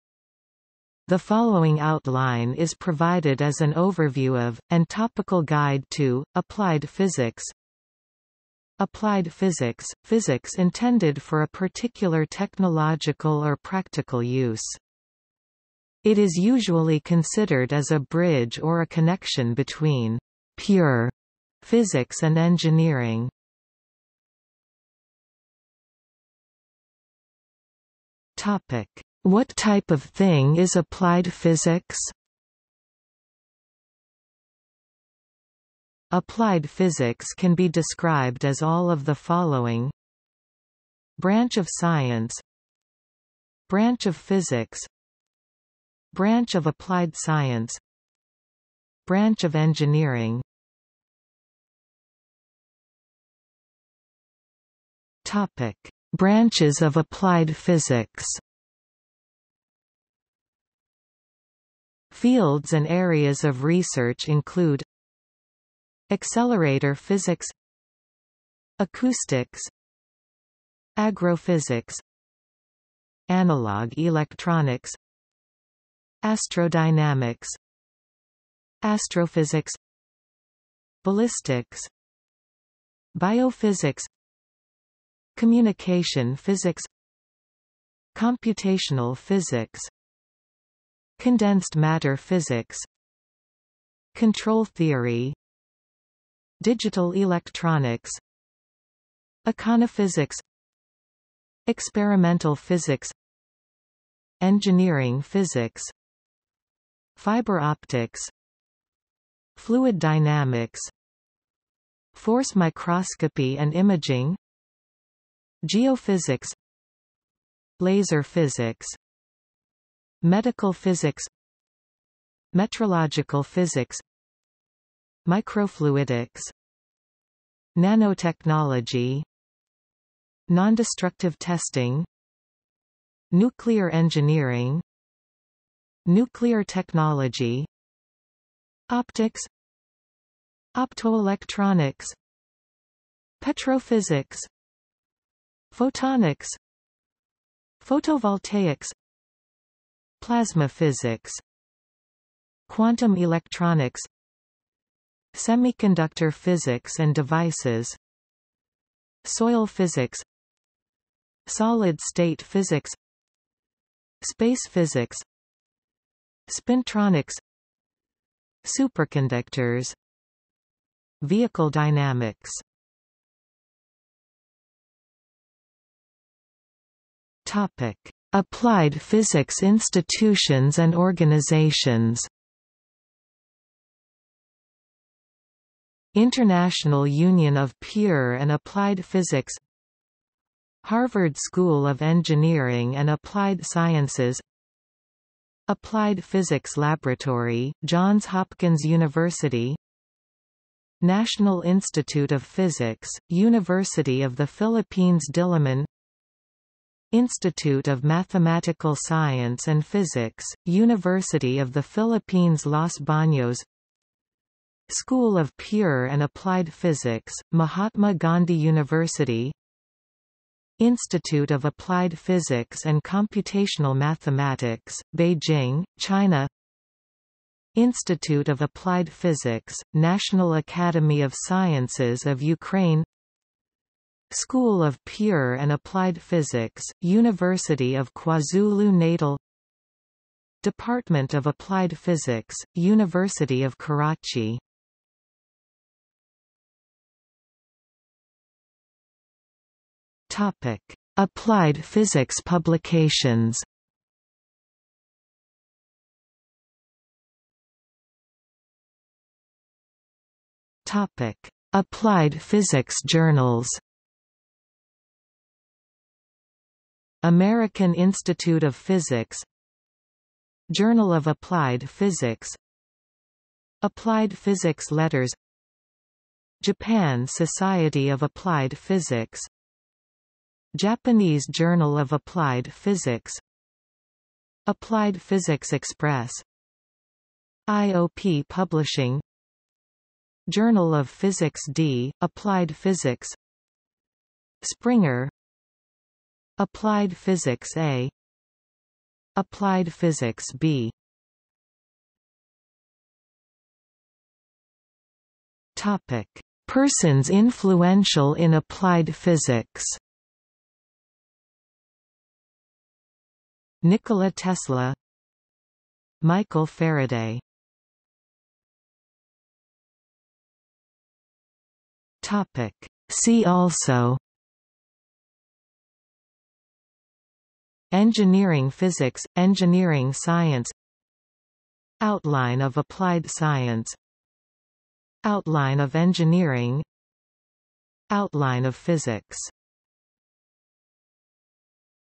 Outline of applied physics | Wikipedia audio article
00:00:24 1 What itype/i of thing is applied physics?
00:00:51 2 Branches of applied physics
00:02:53 3 Applied physics institutions and organizations
00:04:06 4 Applied physics publications
00:04:17 4.1 Applied physics journals
00:05:02 5 Persons influential in applied physics
00:05:18 6 See also

- Socrates

SUMMARY
The following outline is provided as an overview of, and topical guide to, applied physics:
Applied physics – physics intended for a particular technological or practical use.
It is usually considered as a bridge or a connection between "pure" physics and engineering.